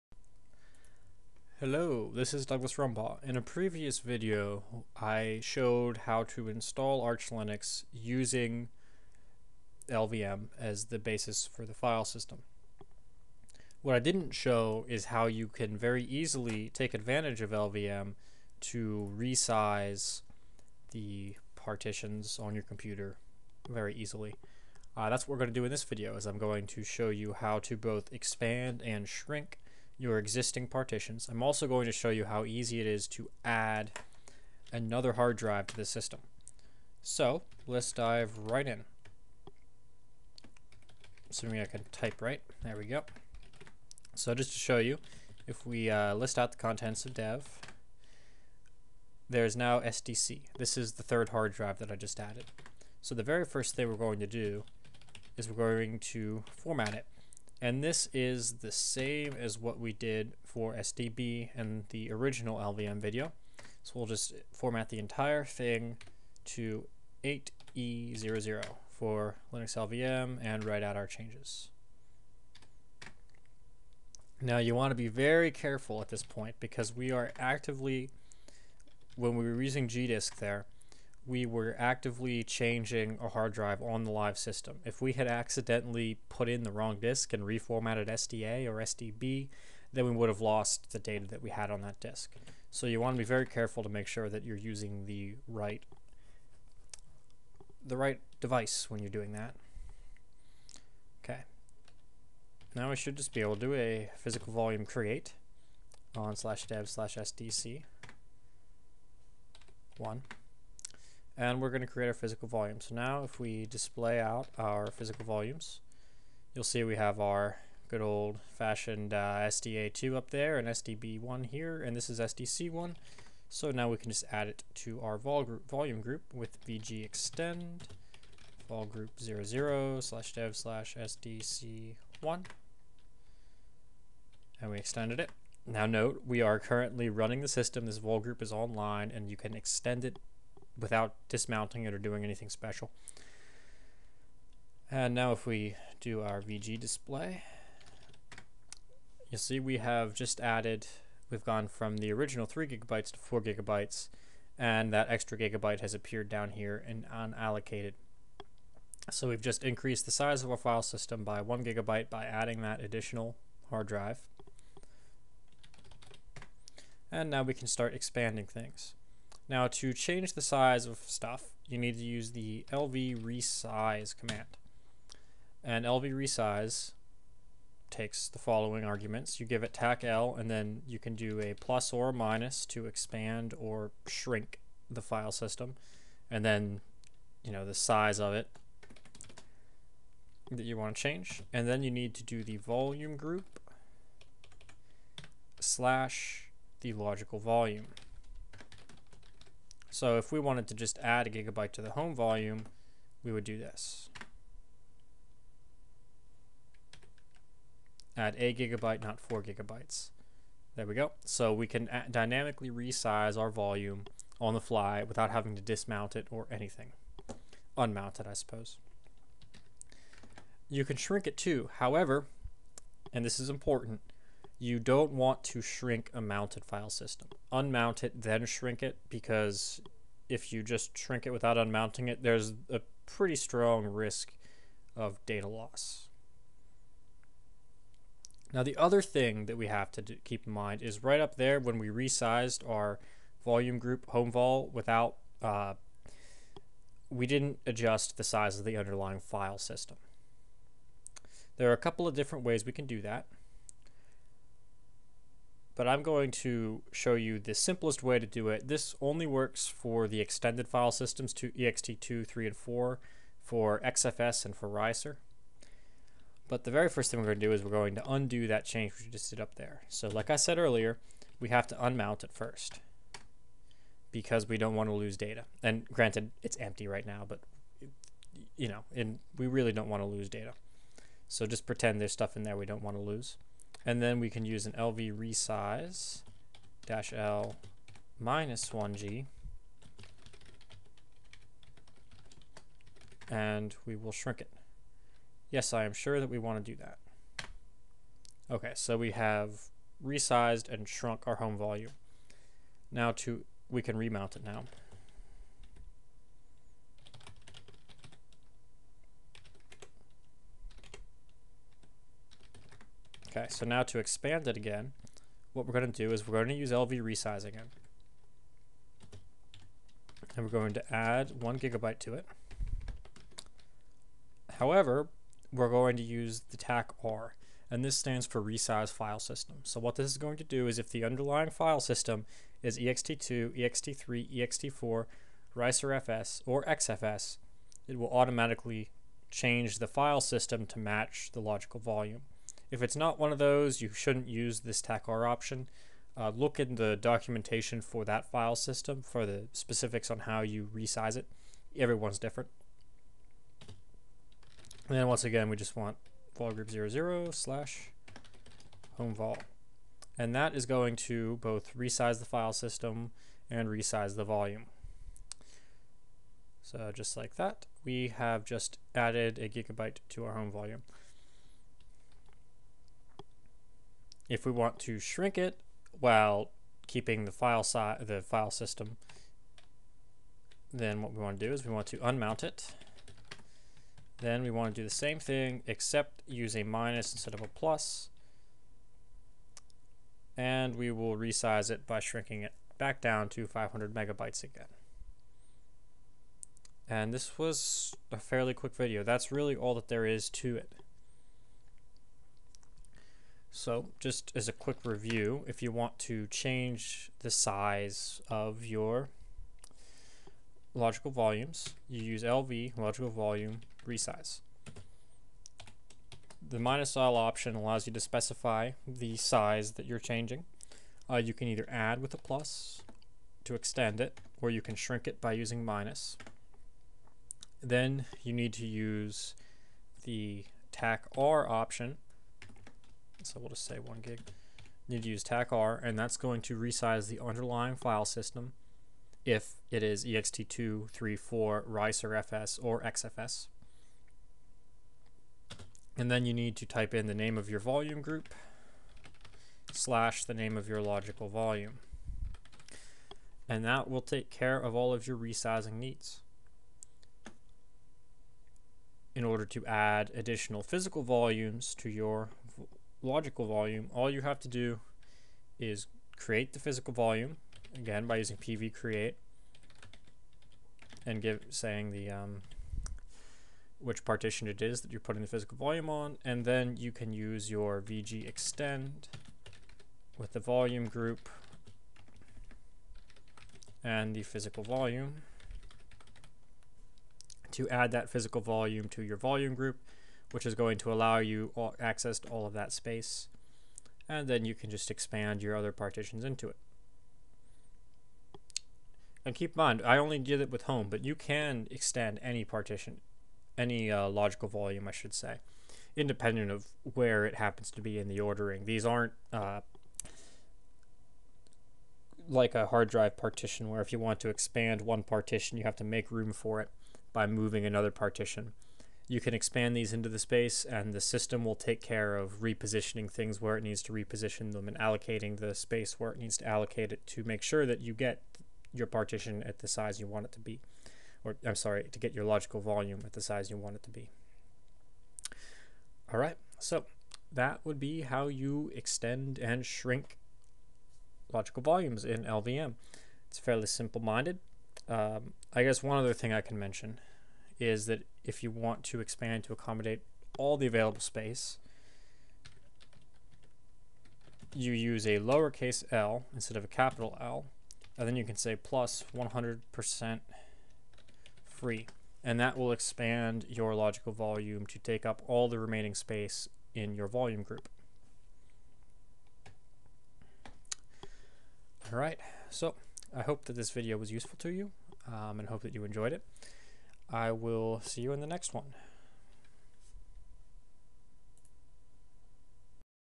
Introduction to LVM Part 2: Resizing Logical Volumes
In which I discuss the use of the lvresize and vgextend commands to change existing LVM systems by adding new hard disks (or other storage devices) and expanding the existing logical volumes into the new space.